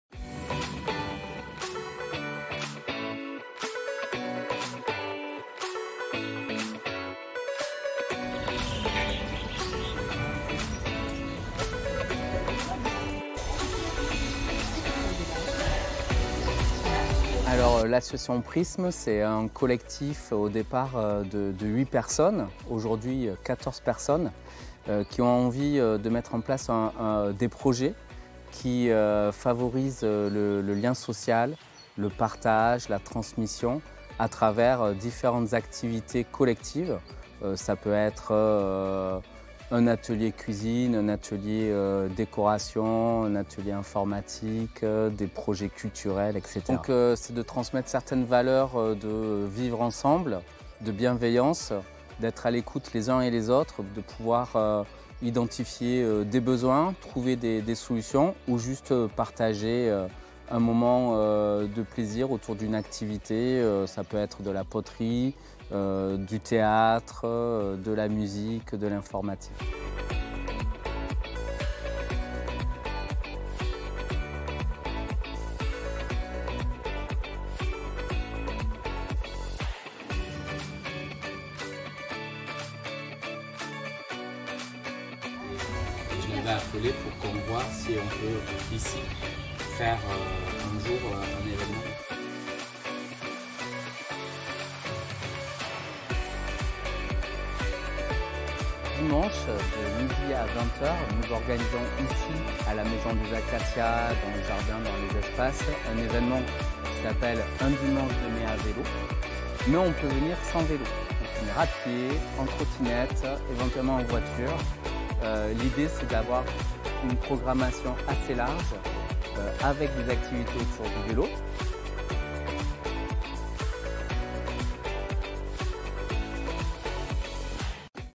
L'Association "Prisme" vous accueille à Audenge
L'Association Prisme est un collectif et une Association fondateur de "La Maison des Acacias" à Audenge.